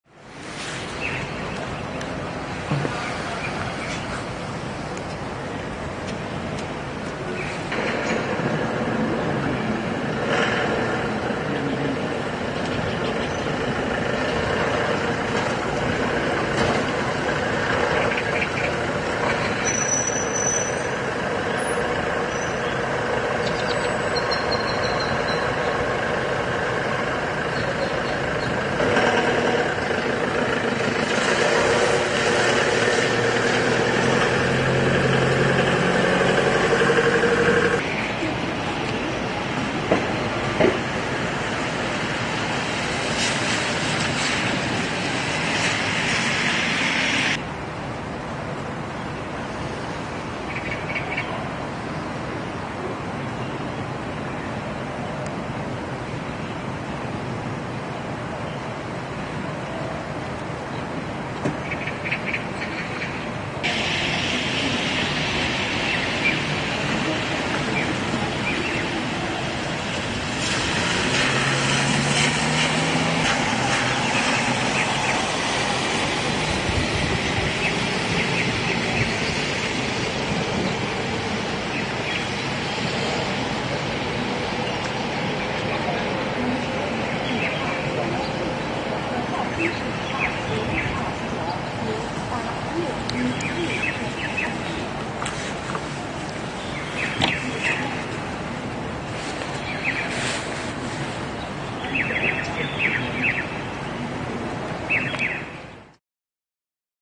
Virus outbreak: Images of Wuhan Medical Treatment Center | AFP
Images of the Wuhan Medical Treatment Center on Friday morning as Chinese authorities rush to contain a deadly SARS-live virus outbreak. IMAGES